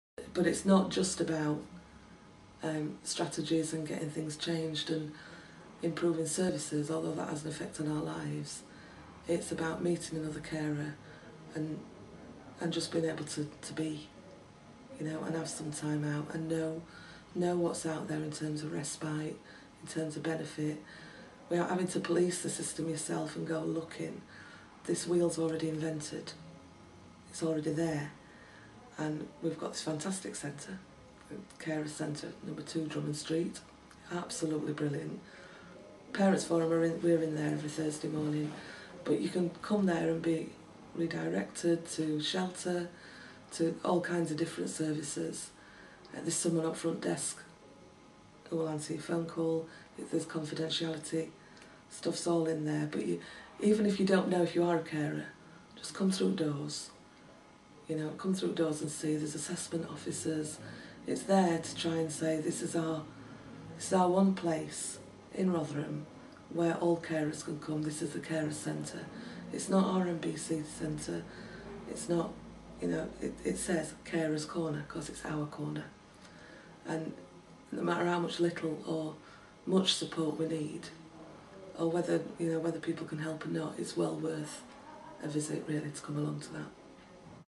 Carers Corner & Carers Forum the first step to support for carers in Rotherham
About the service for carers in the centre of Rotherham called Carers Corner which will put you in contact with other carers